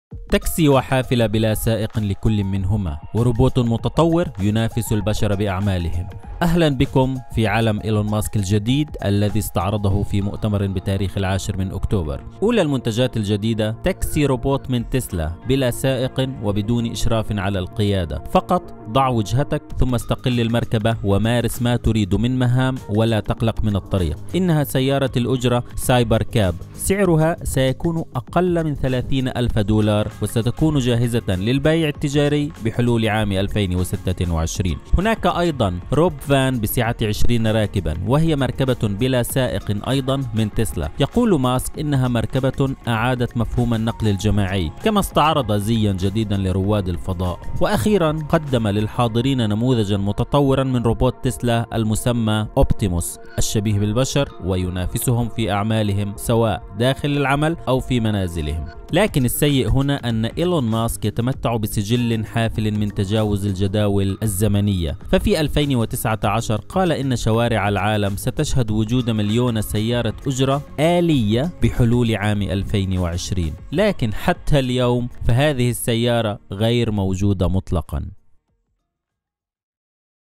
إيلون ماسك يعرض أحدث اختراعاته
تكسي وحافلة بدون سائق وروبوت ينافس البشر بأعمالهم.. تعرفوا على أبرز ما جاء في مؤتمر إيلون ماسك